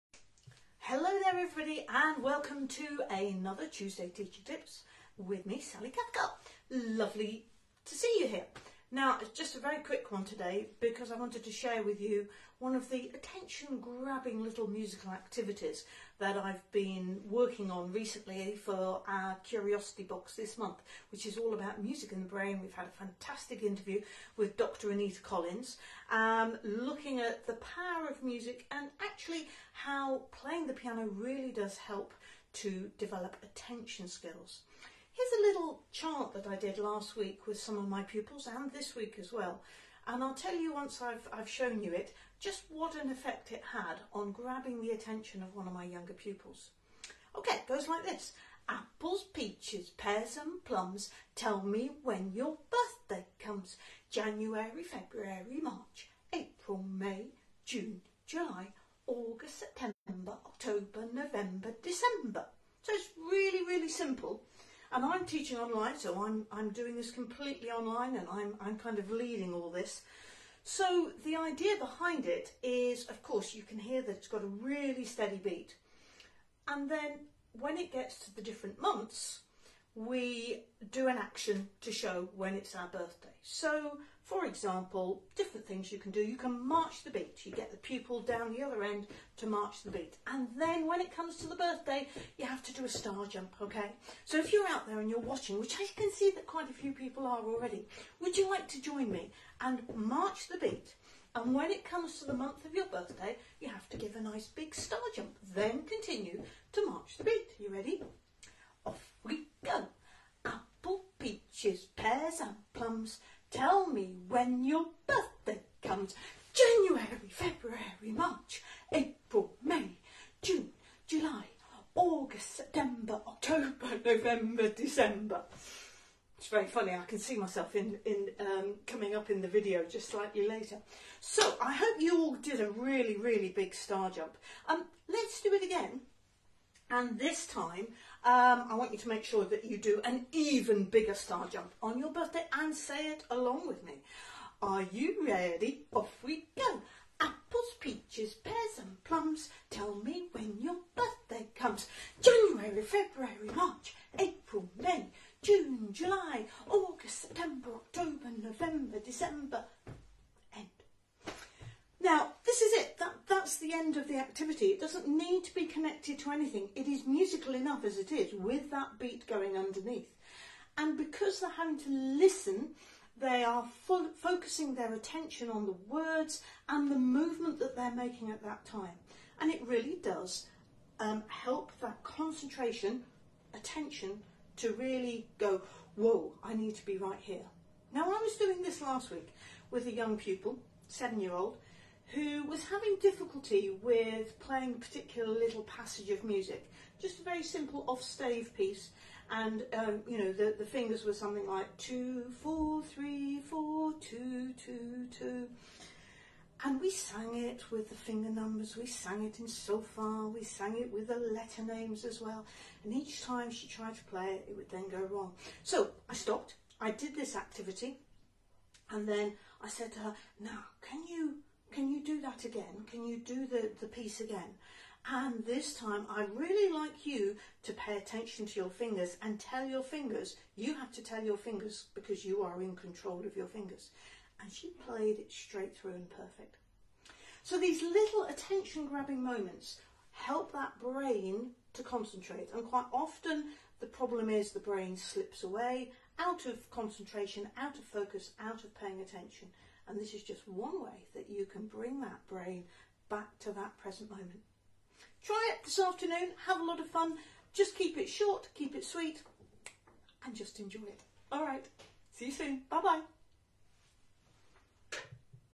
Tuesday Teaching Tips - grabbing their attention and engaging the brain
The focus of our October Curiosity Box is music and the brain. Sally shares one of the ideas for quickly helping students to refocus.

Membership of The Community is currently free for the first month. After that, the normal subscription applies. We’d love to see you there.